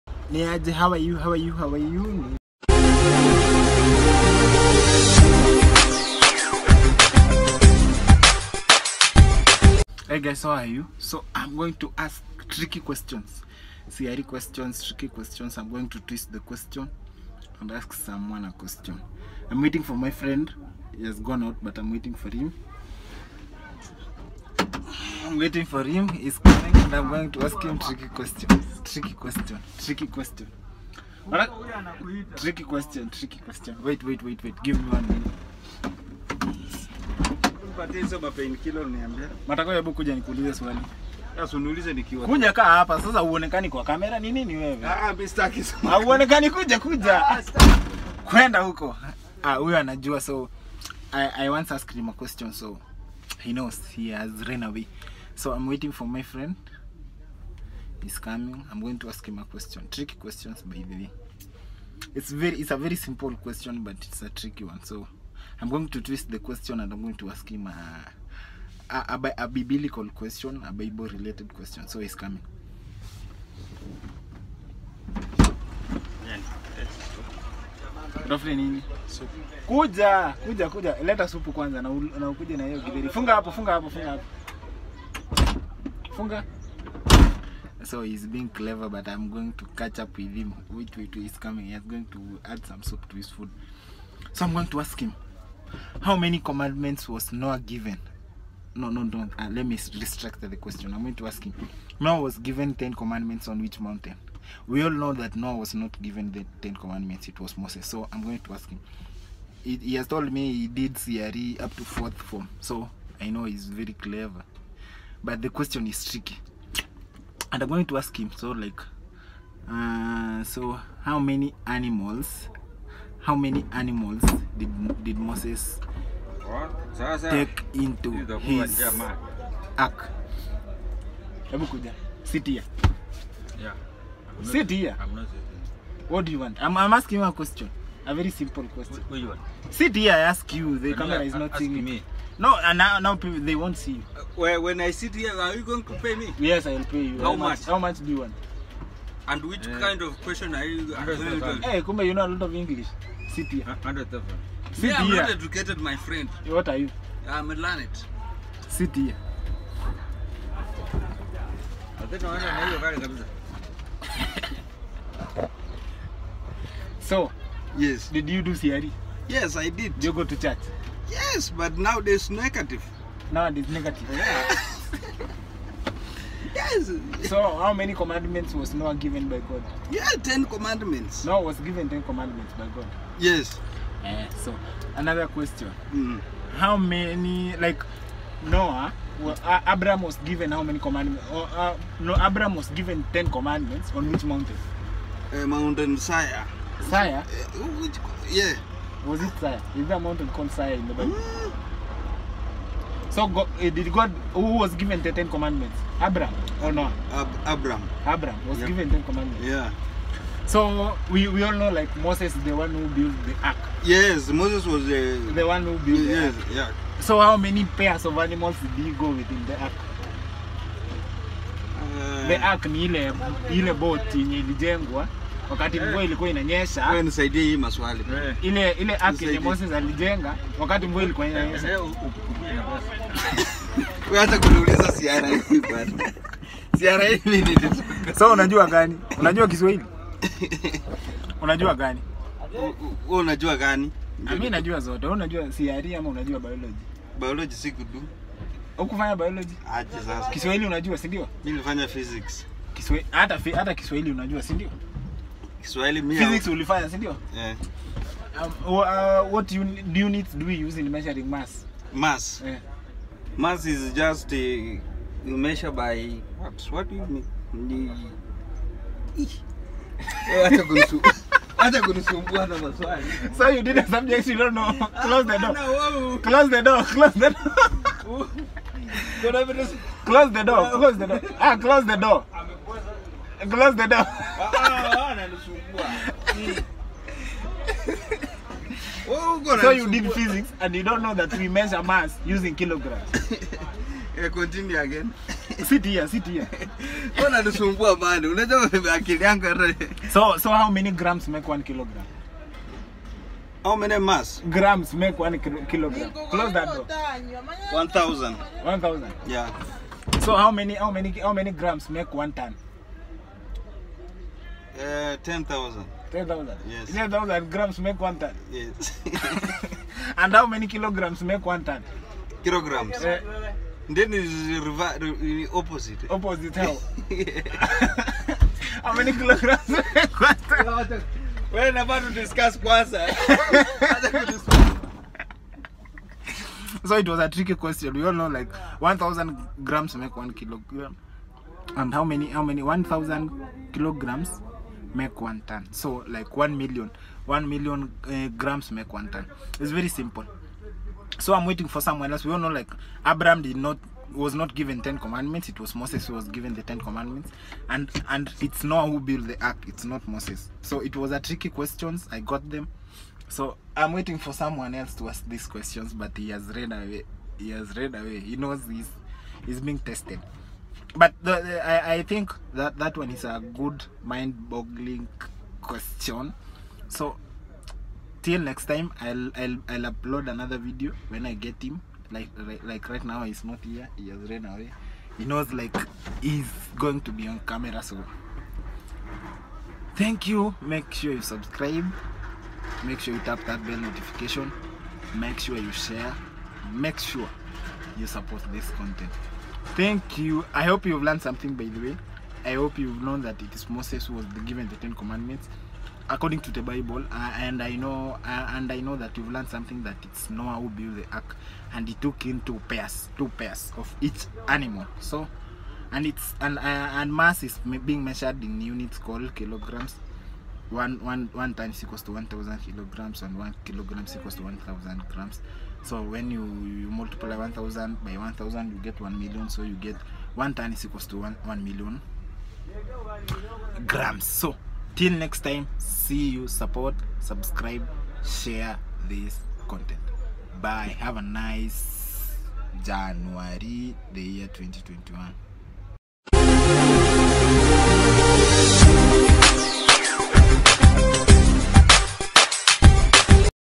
Strt quiz -Abraham and Noah were given how many Commandments??; on which mountain???
New segment of street questions...😂 😂 let's learn something...😂 simple and easy question...😂 😂 who knows the answer???😂😂😂